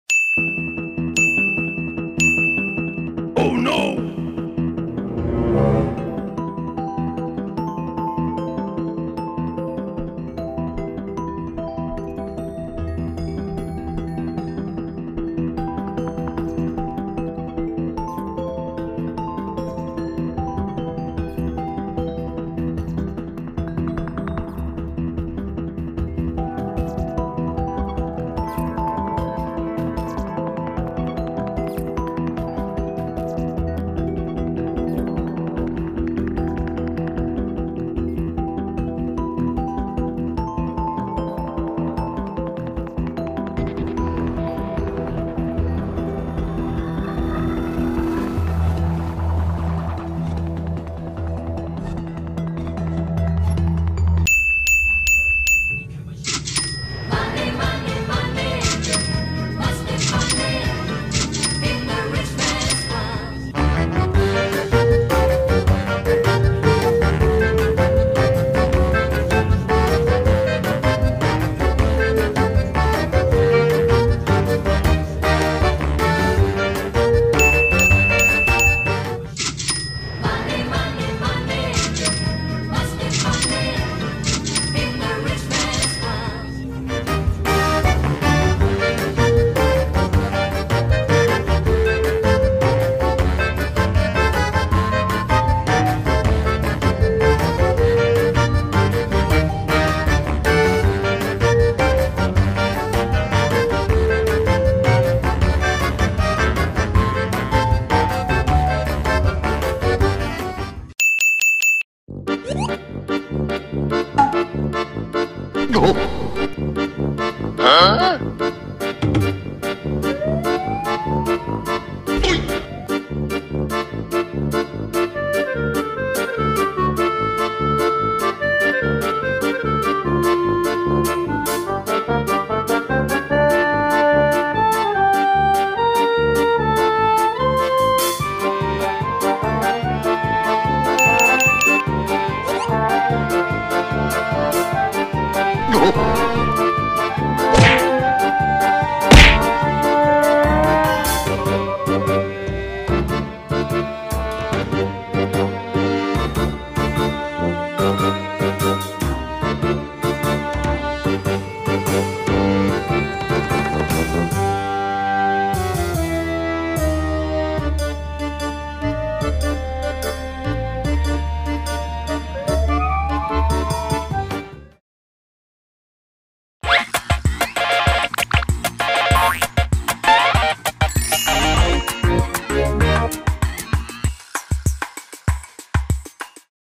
三寶騎士路口突衝出 他嚇壞急煞差一秒相撞｜路口未減速｜危險駕駛｜三寶相遇
☛看更多

☑影片授權：FB／八卦2村 - 行車紀錄器影片上傳中心